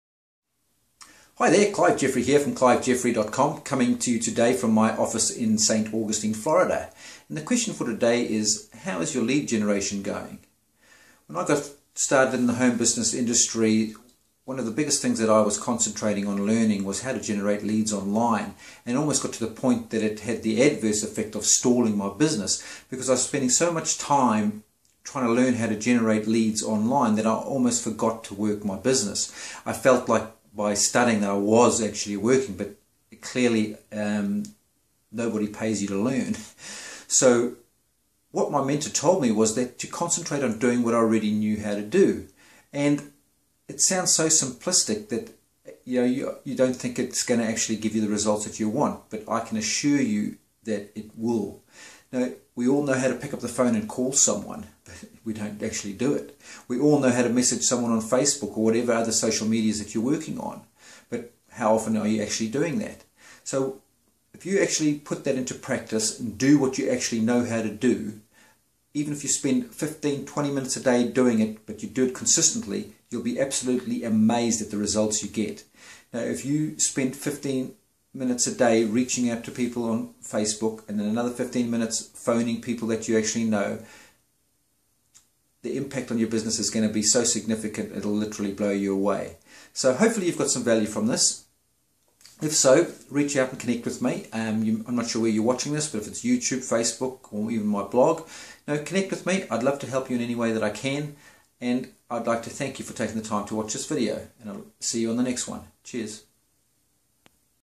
Do What You Already Know How to Do
Simply Do What You Know How To Do
Not sure if this rings any bells with you or not?
But when I first got started learning to generate leads
for my home business online, I was in unchartered waters!
I had done a bit of research and started trying to learn all I could
The more I learned, the more I realized that I didn’t know!
Sound familiar?
Before too long, I found myself doing more and more studying. Learning this strategy and that, literally learning just enough about each strategy to be dangerous!
The more studying I did, the less actual work I did on my business.
My results stagnated, my frustration grew and those voices started to occupy my head. You know the ones, the insidious voice of doubt.
It was then that I was lucky enough to stumble across my mentor!
(If you don’t have a mentor, get one!)
What a breath of fresh air.
He told me to step back from all the studying for the moment and simply do what I already knew how to do.
It’s hard to believe that such a simple statement could have such a big impact on my business but it did!
We all know how to use a phone but how many of us do on a consistent basis?

Now I don’t mean answer it when it rings either!
I mean that you must set up a daily allotment of time where you actively call and prospect people. EVERY DAY!
How long do I have to do that for, I hear you ask (I asked the same question myself!)
The answer is “You DO it Until!”
Same goes for prospecting on Facebook or any other form of social media. Do you set aside time each day to connect with new people?
If not, why not?
If you spend 15 minutes a day on each of these two activities every day for 30 days, you would be stunned at the results you would achieve!
If you have any questions at all or would like help to implement these strategies give me a call on I would love to assist in any way that I can.
Cheers
Clive

PS. You should see this awesome CRM I use to keep track of all my leads, never lose track of your contacts again, check it out HERE

Sound familiar?

Cheers
Clive